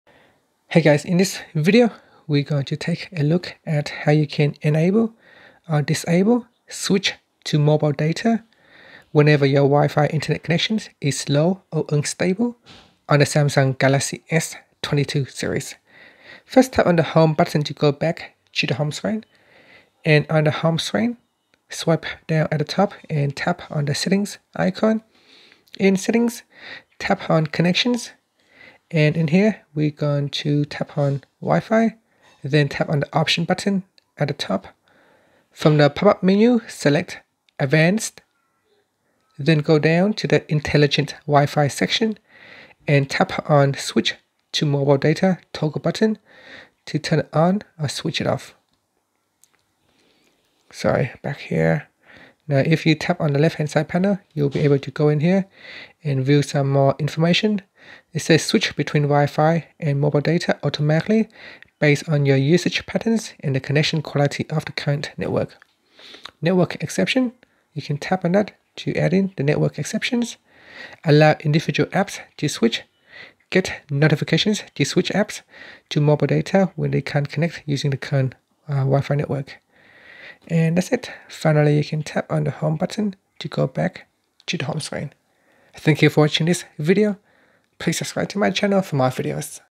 Galaxy S22/S22+/Ultra: How to Enable/Disable Switch to Mobile Data
Learn how you can enable or disable switch to mobile data on the Samsung Galaxy S22 / S22+ / S22 Ultra.